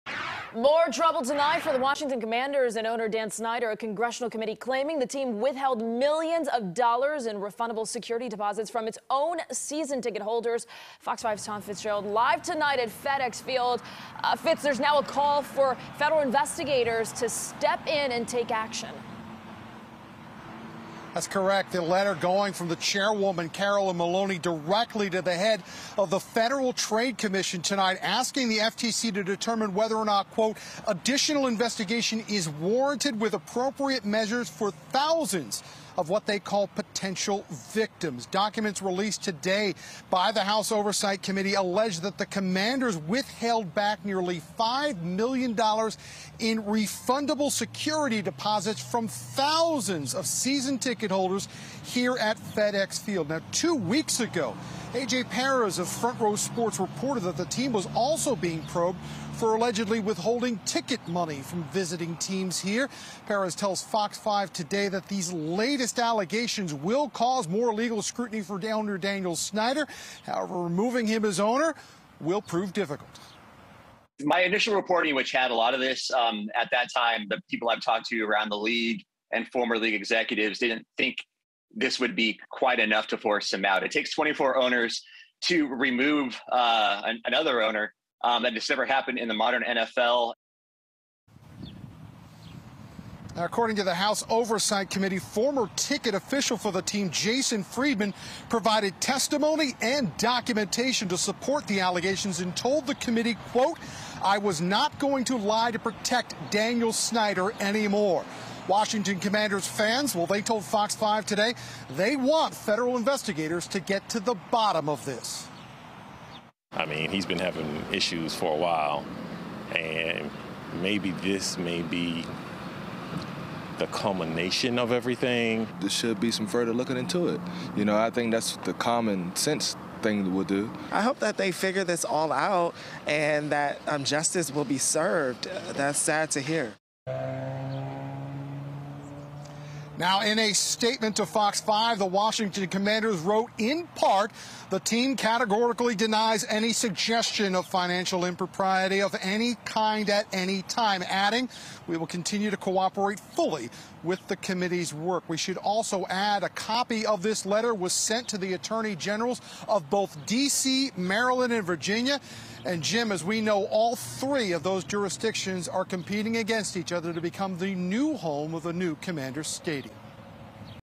Washington Commanders may have engaged in ‘unlawful’ financial conduct, says Congress to FTC
Members of Congress have sent a letter to the Federal Trade Commission saying senior Washington Commanders’ executives, including team owner Dan Snyder, may have engaged in potentially unlawful financial conduct, affecting thousands of fans and the NFL.